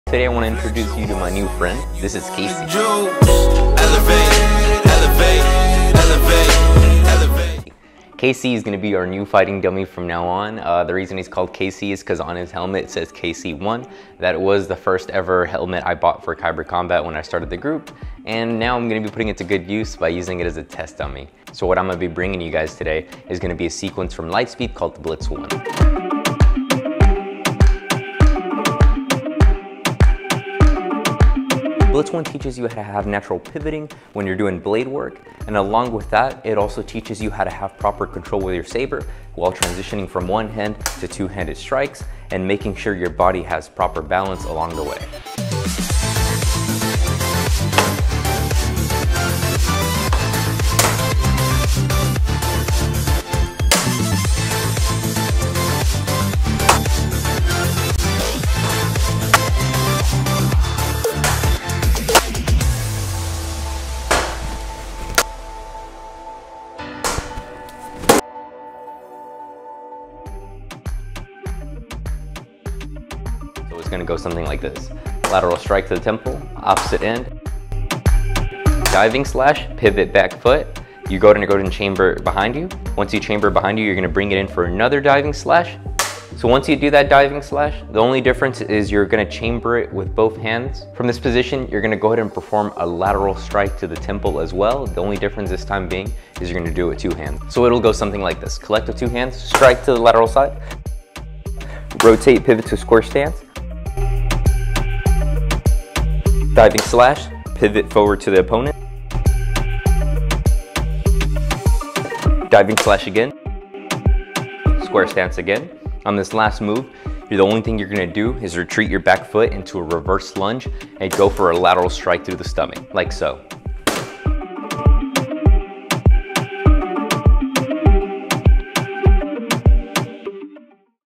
BLITZ 1 LIGHTSABER COMBAT SEQUENCE FT/ KACEY (KC1)
BLITZ 1 is a Beginner Lightsaber Combat Sequence. it works on pivoting and blade control in transitions. Highly recommended to pick this up it's simple and gives you great fundementals!